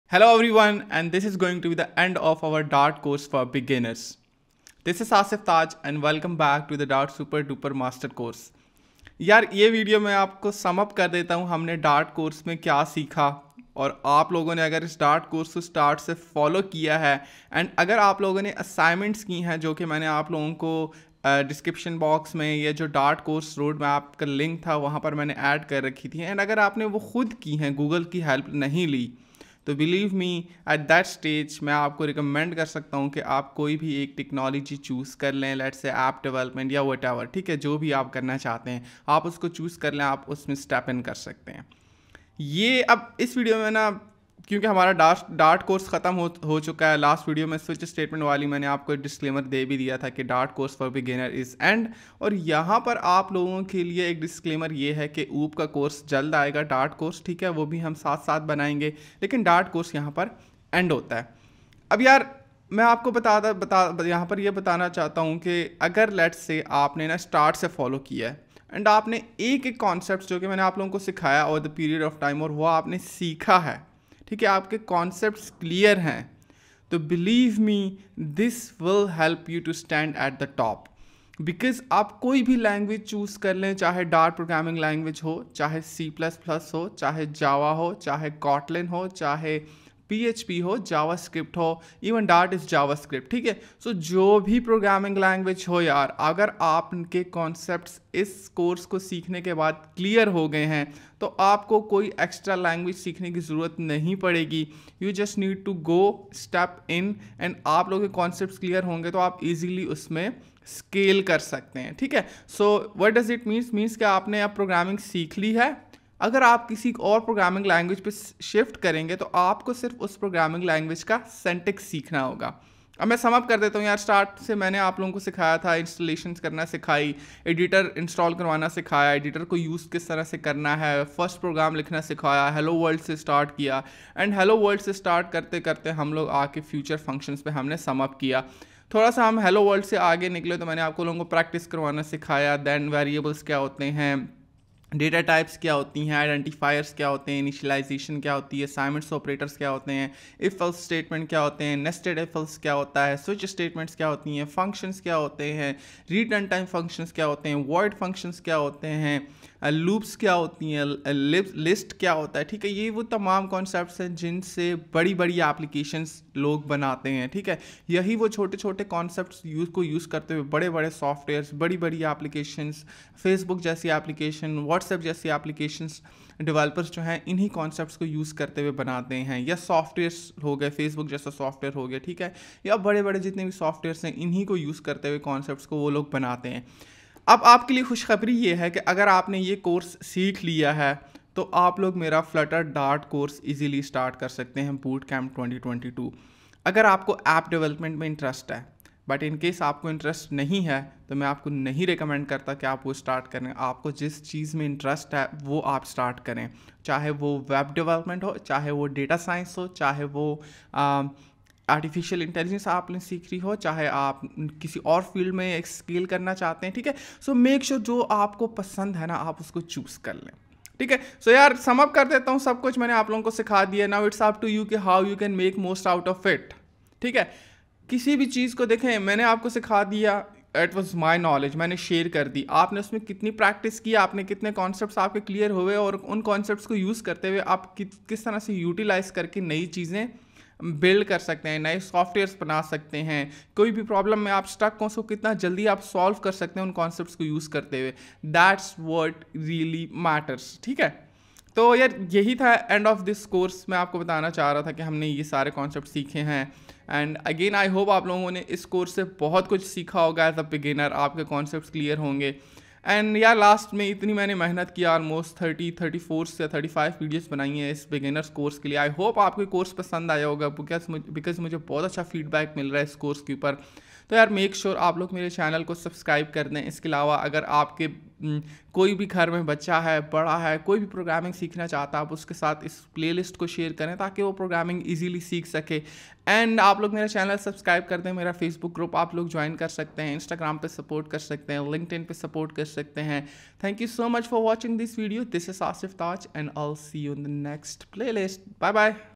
End Of Dart Course || Dart Programming Tutorials in Hindi/Urdu
In this video i will sum-up what we have learned in this course.

dart functions dart async functions dart future functions, dart programming tutorials async await future